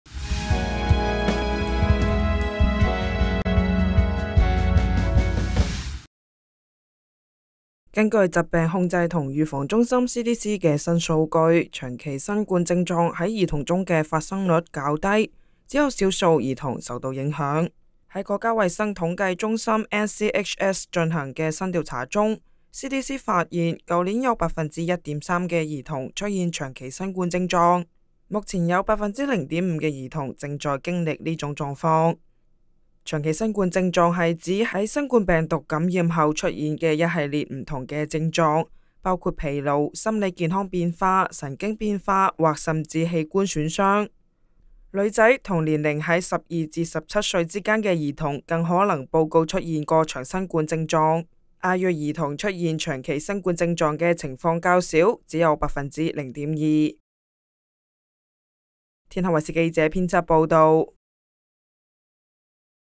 【健康快訊】健康: CDC數據顯示 兒童長新冠發生率低 CDC reveals small number of children experiencing long COVID
根據疾病控制和預防中心CDC的新數據，長期新冠症狀在兒童中的發生率較低，只有少數兒童受到影響。
在國家衛生統計中心NCHS進行的新調查中，CDC發現，去年有1.3%的兒童出現長期新冠症狀，目前有0.5%的兒童正在經歷這種狀況。
長期新冠症狀是指在新冠病毒感染後出現的一系列不同的症狀，包括疲勞、心理健康變化、神經變化或甚至器官損傷。
女孩和年齡在 12-17 歲之間的兒童更可能報告出現過長新冠症狀。亞裔兒童出現長期新冠症狀的情況較少，只有0.2%。